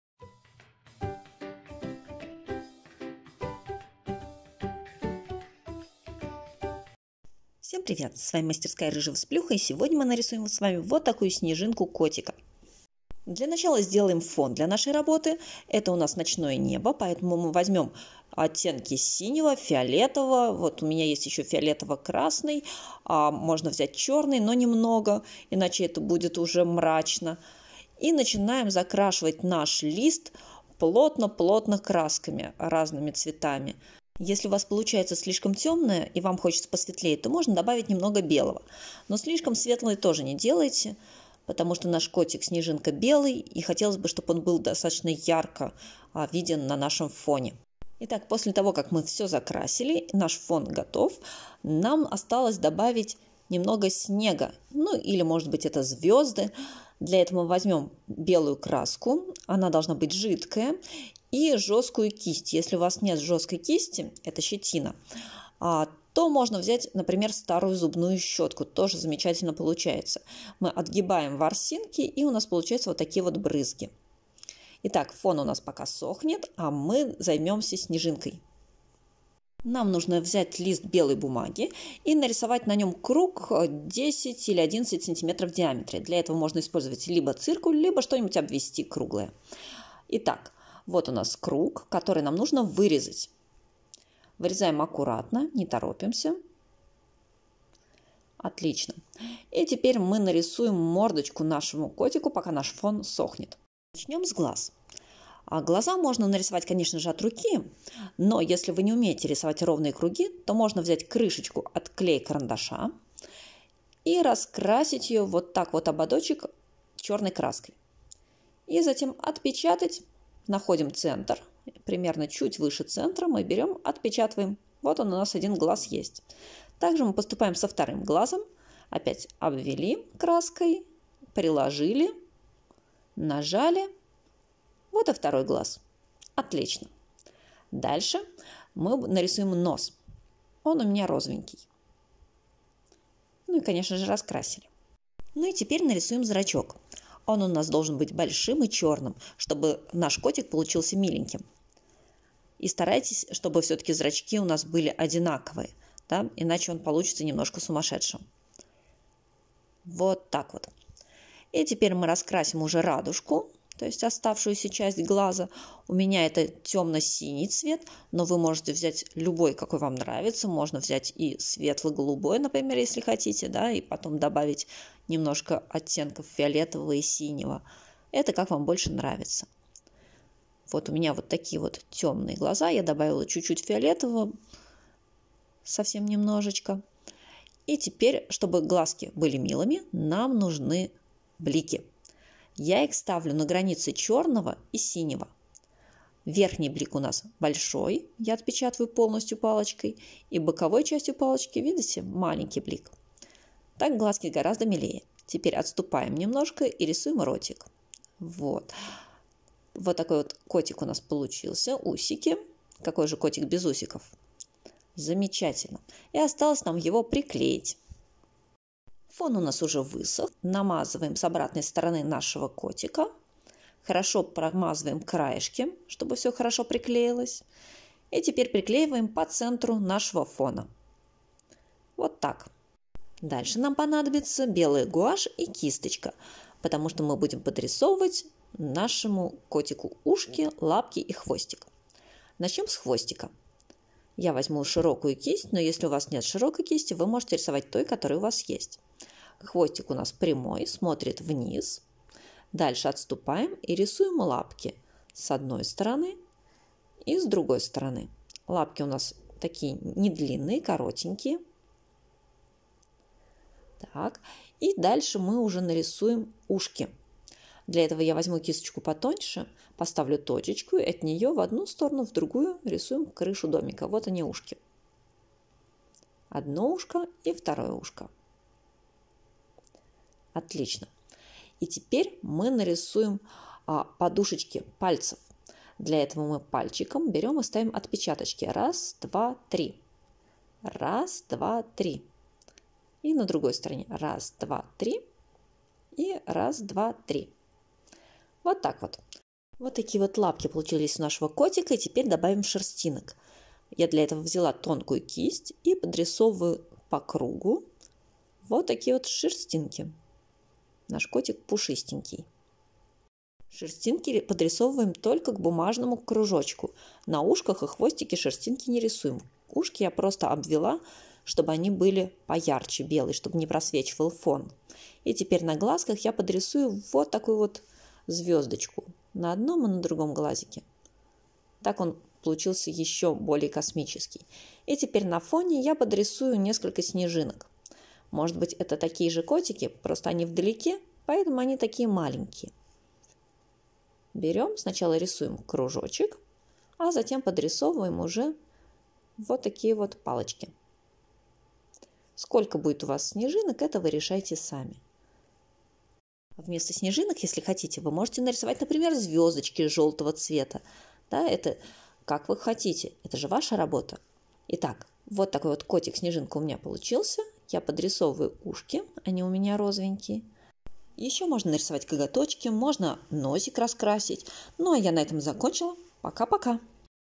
Мастер-класс "Рисуем с детьми котика-снежинку"
На этом мастер-классе я покажу, как легко нарисовать котика-снежинку.

Для работы нам понадобятся:
- лист ватмана или акварельной бумаги,
- малярный скотч (чтобы зафиксировать лист),
- краски гуашь (оттенки синего и фиолетового, черная, белая),
- кисть толстая и тонкая (лучше синтетика или колонок),
- кисть щетина или старая зубная щетка,
- баночка для воды,
- лист белой бумаги альбомный или для принтера,
- циркуль или простой карандаш и что-то, что можно обвести, чтобы получить круг диаметром 10-11 см,
- ножницы,
- клей-карандаш,
- тряпочка или влажные салфетки, чтобы вытереть руки (не обязательно),
- фломастеры.